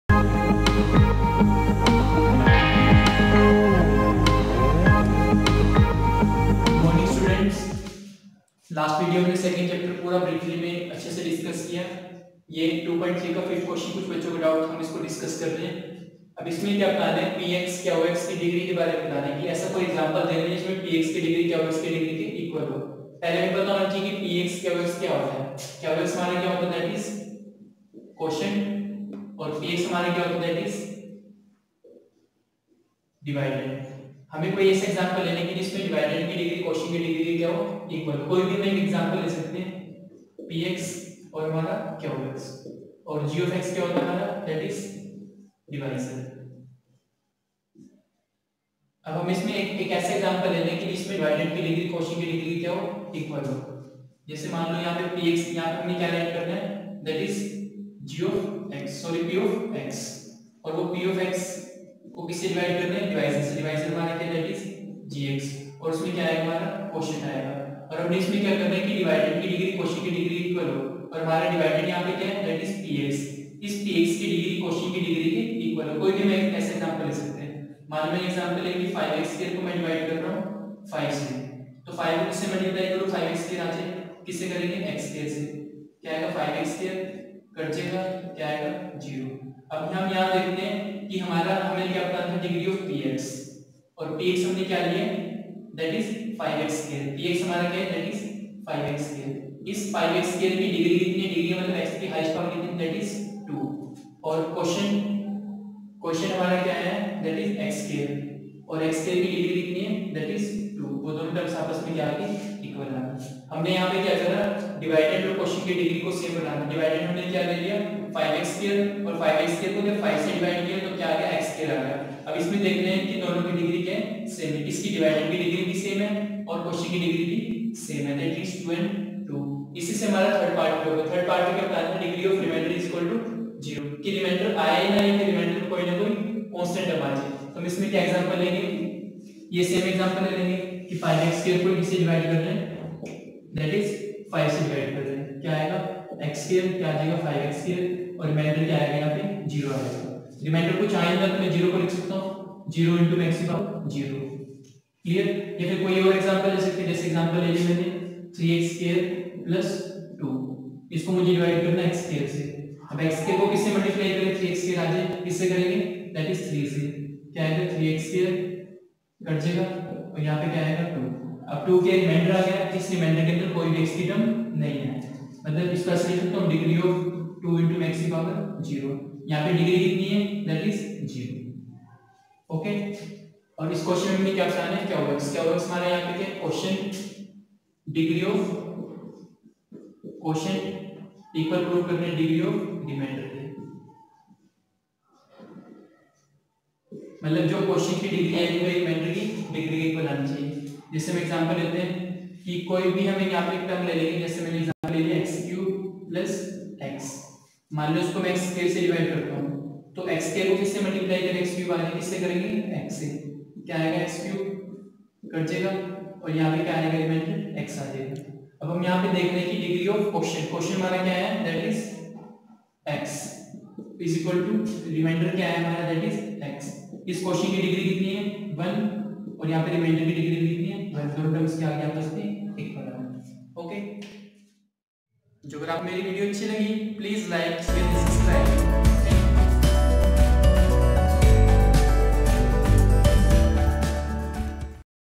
Polynomials |Chapter - 2nd |Question no 5 of Exercise 2.3| Maths NCERT Class - 10 by Bhavnish Wadhwa
Polynomial:CBSE Class-10th Chapter-2nd
Hello students,here is this video we are going to do the Question no 5 of exercise 2.3